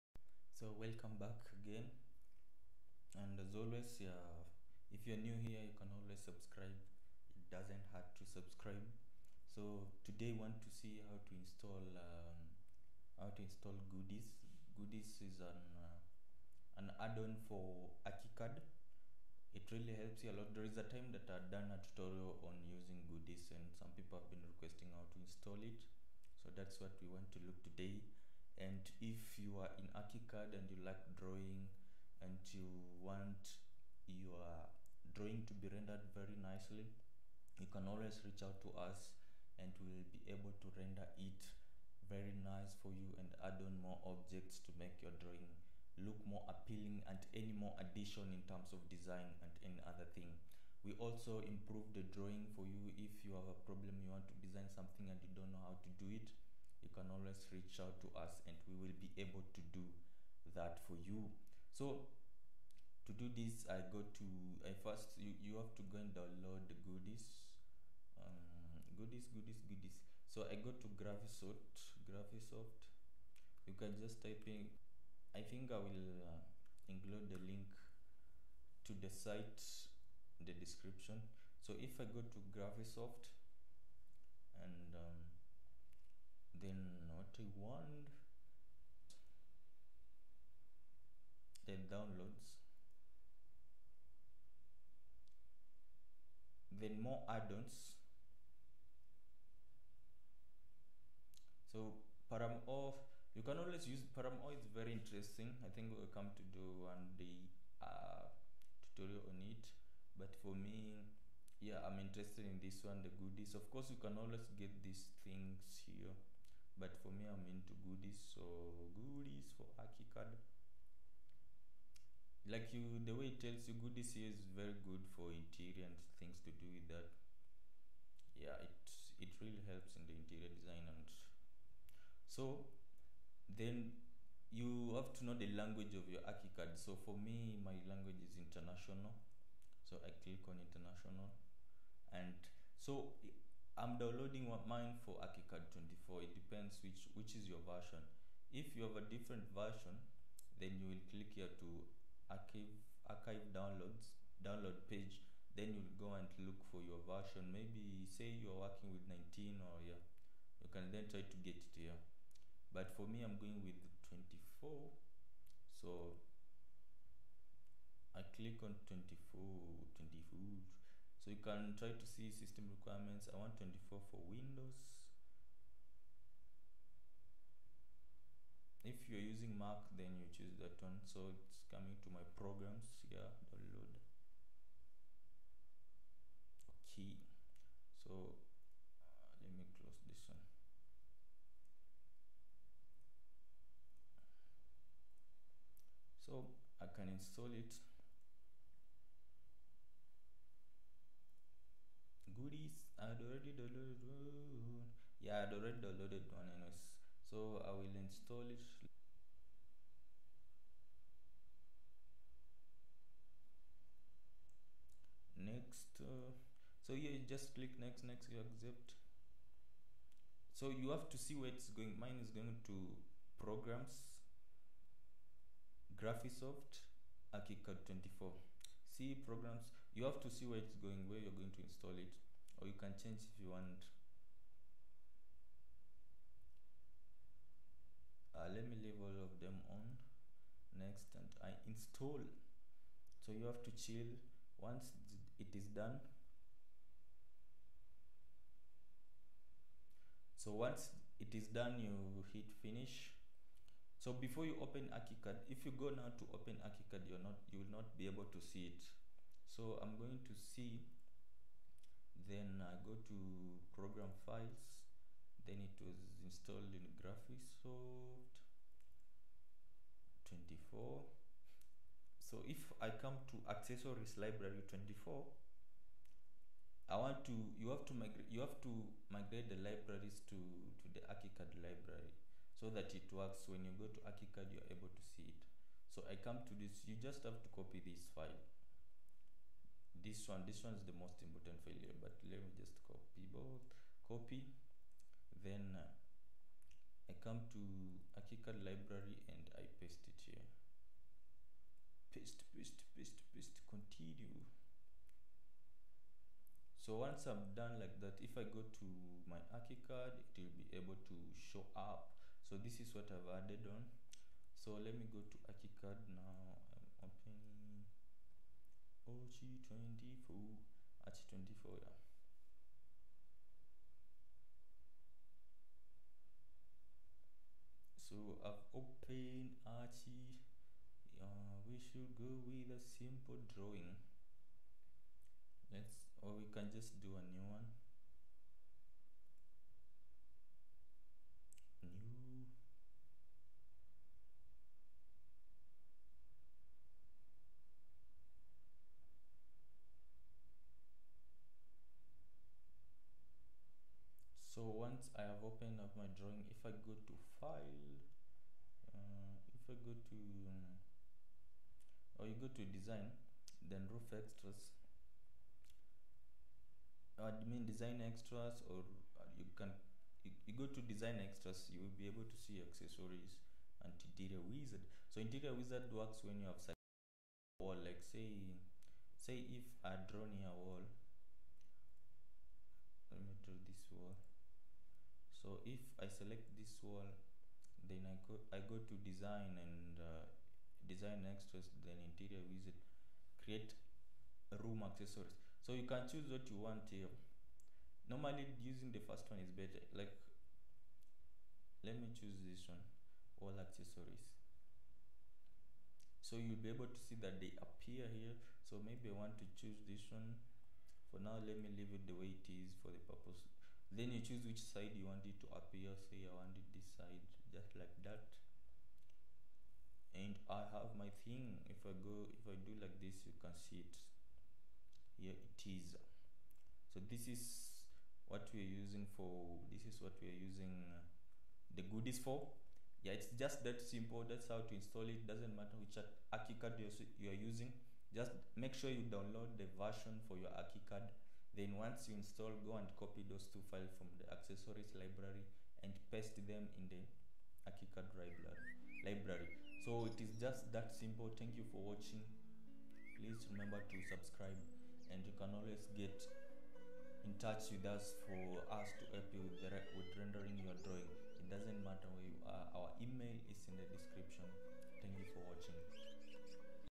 How to Install Goodies in ArchiCAD and Use Goodies in ArchiCAD
Goodies is an add-on that is very helpful in interior design, colour schemes and simulation of the construction process among many other uses. In this video we show you how to install goodies in your ArchiCAD